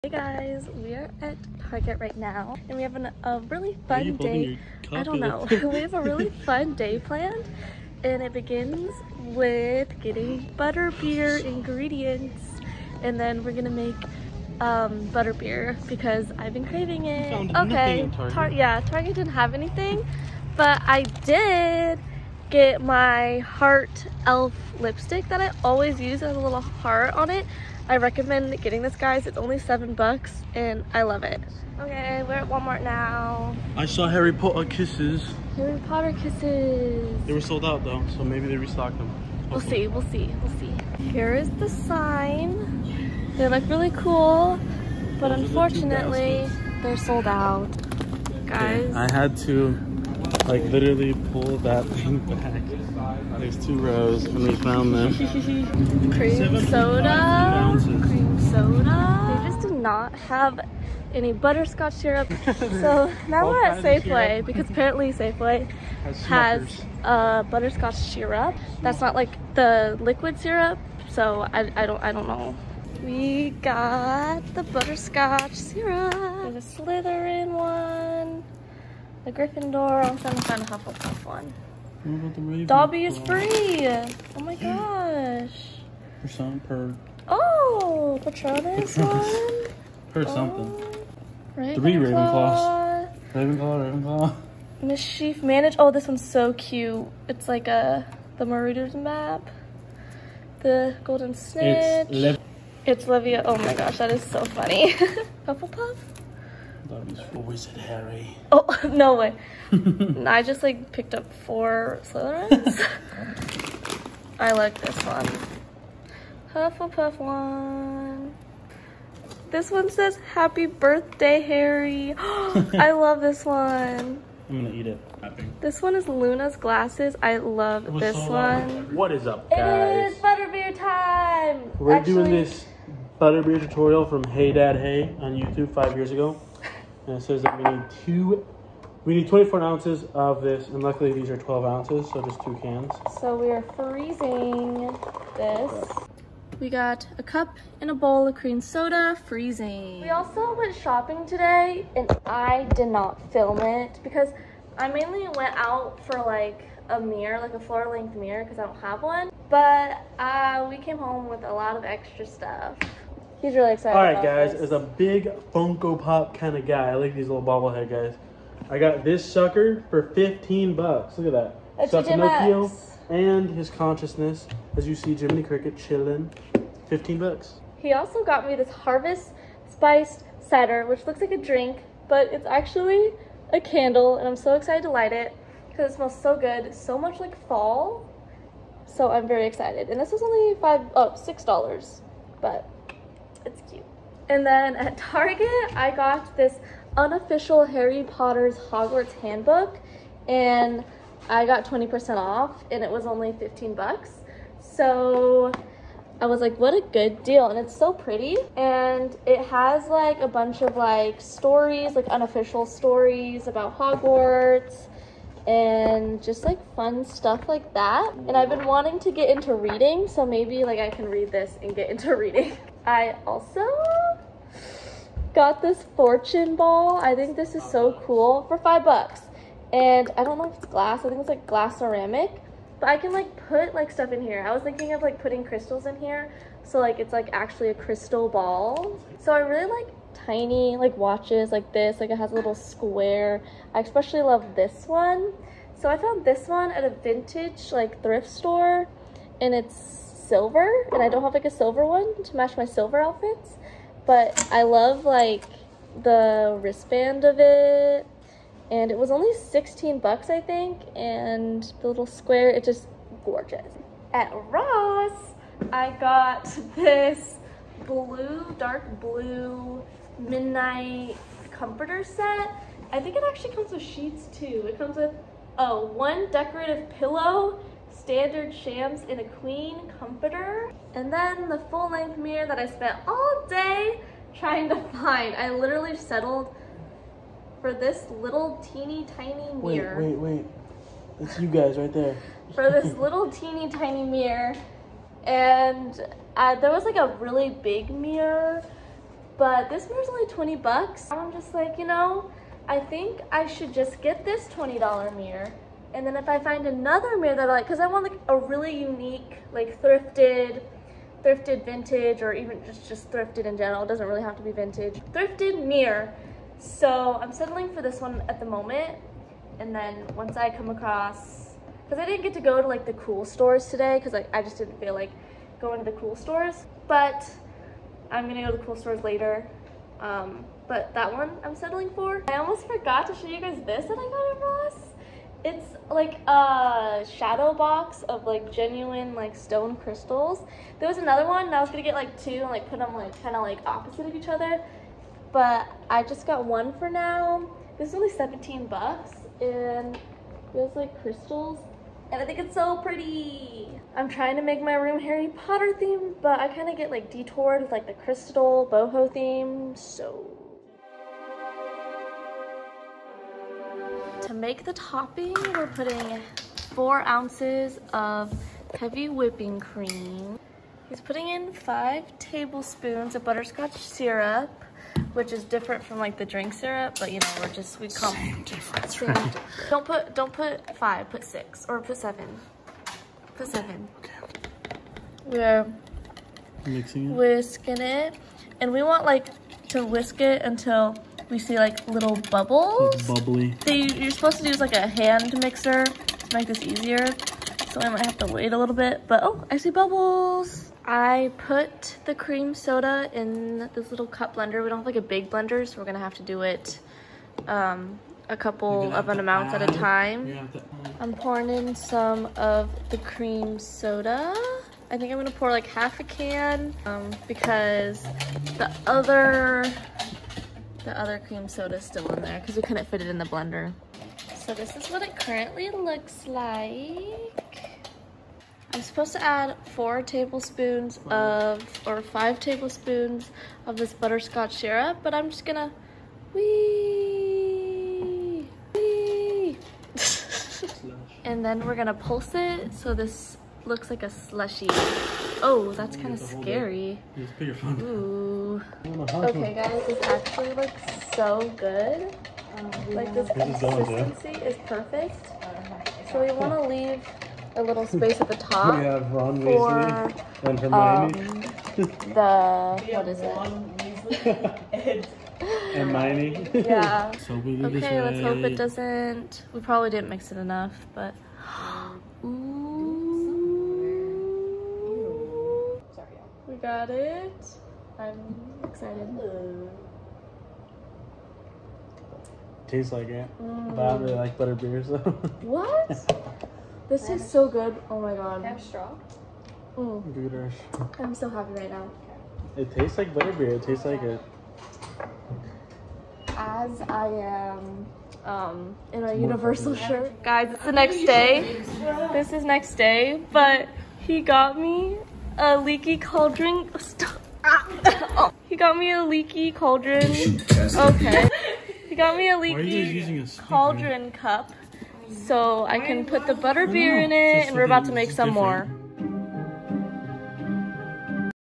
How to Make Butterbeer at Home!🪄(easiest recipe ever!)
I am so happy (and addicted)